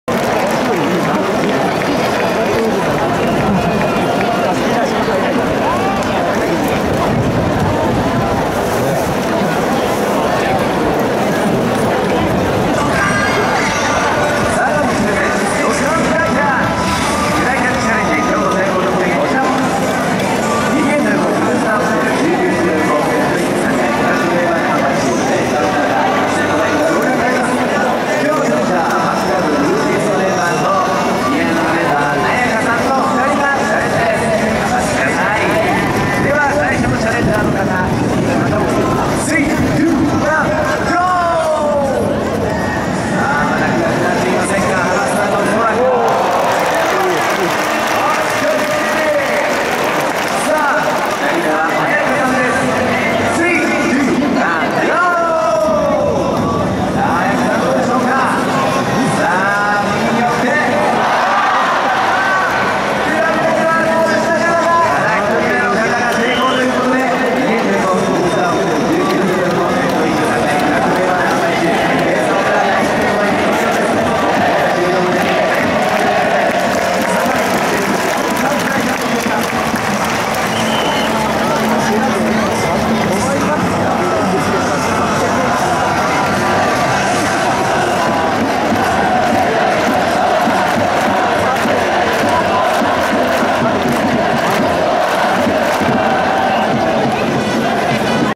【がんばれ！Ayakaちゃん(diana 2024)】フライキャッチがめっちゃ惜しい！ 7/10 YOKOHAMA STAR☆NIGHT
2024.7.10
Ayakaちゃんがチャレンジしたイニング間イベント『ドッカーン！FLYCATCH』です。今回はめっちゃ惜しかったです。
次はがんばれ！Ayakaちゃん

【新メンバー】ディアーナ(diana)今シーズン初ステージ(横浜DeNAベイスターズ チア)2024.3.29開幕戦